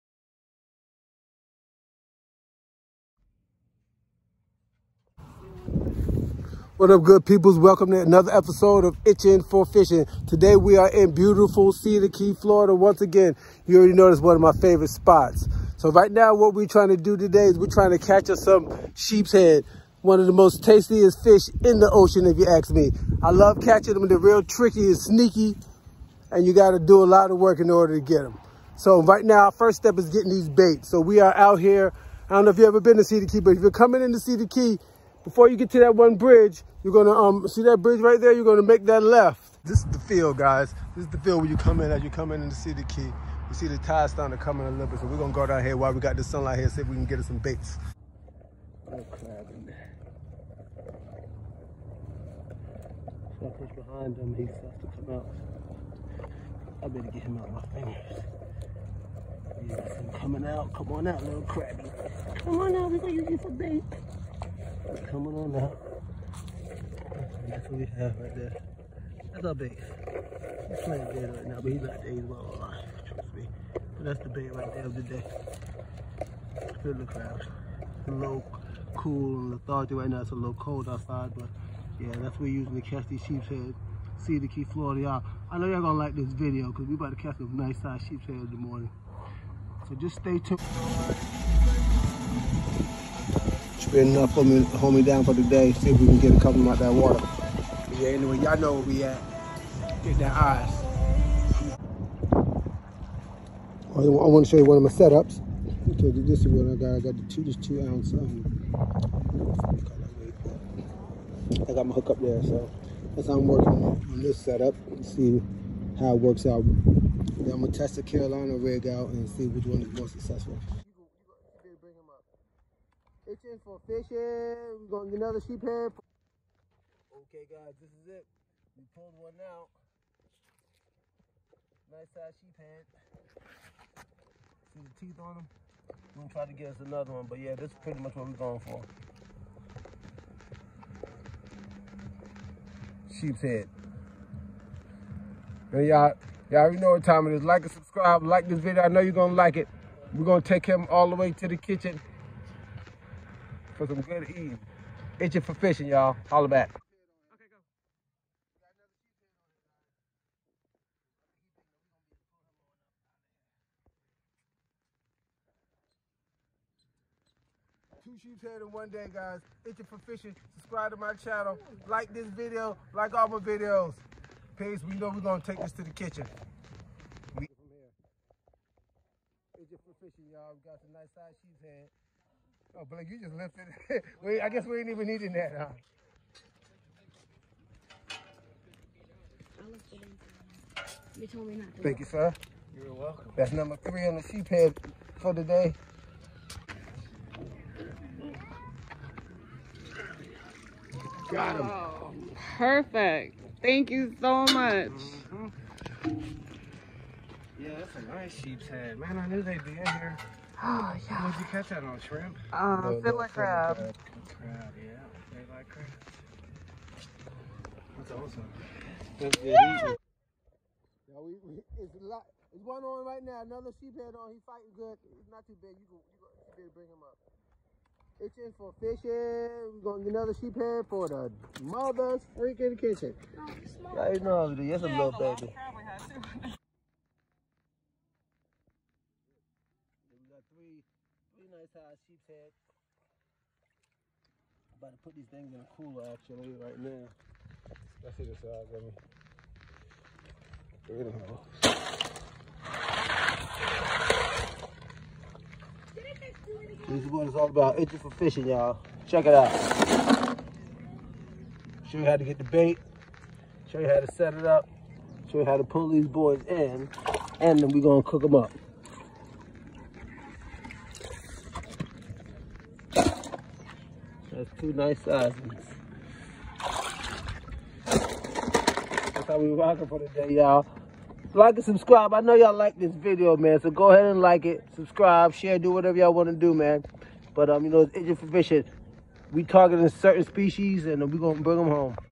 Fishing tips( How to catch Sheepshead in Cedar Key Fl)
This video is a complete guide to a successful fishing trip to Cedar Key, Fl